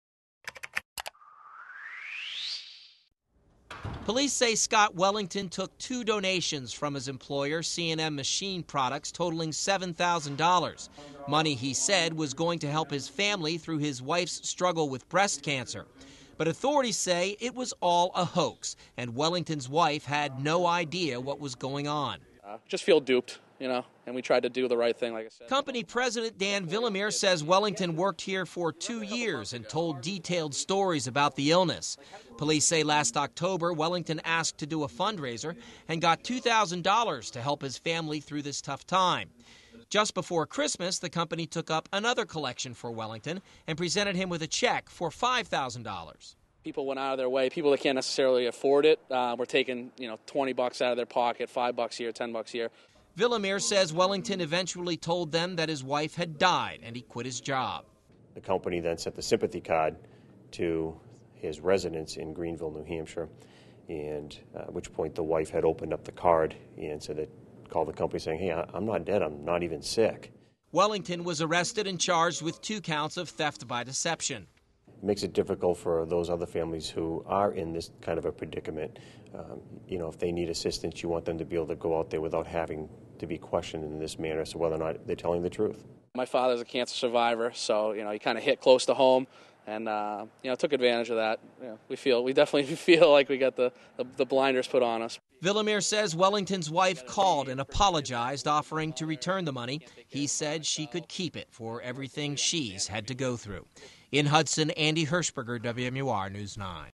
Man Accused Of Faking Wife's Illness, Death
The Hudson Police Department arrests a 31-year-old Greenville man it said lied about his wife's illness and death to get money from his employer.